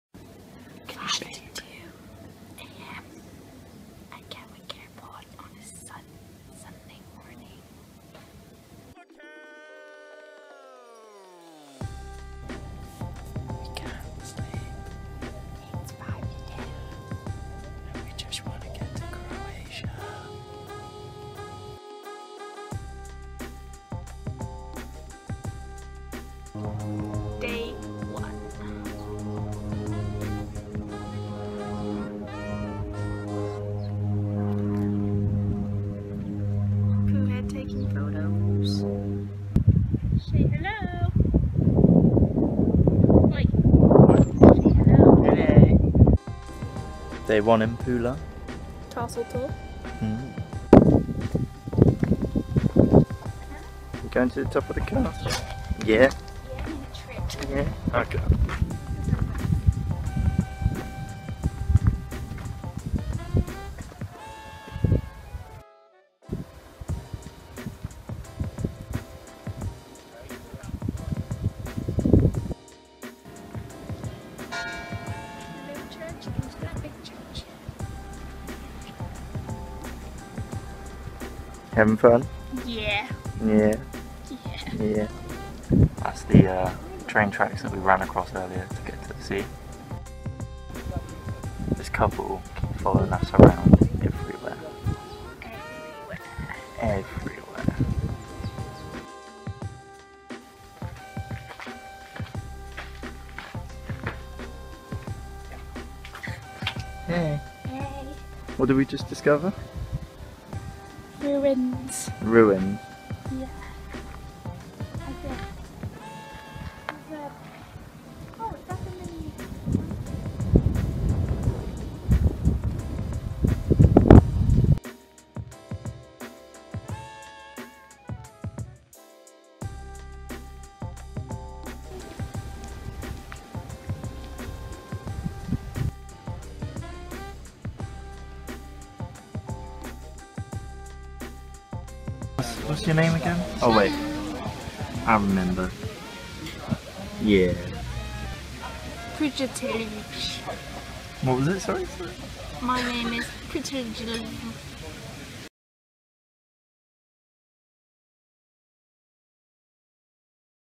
Day one: Pula, Croatia
our first day in Pula (Croatia), sorry for the rough edit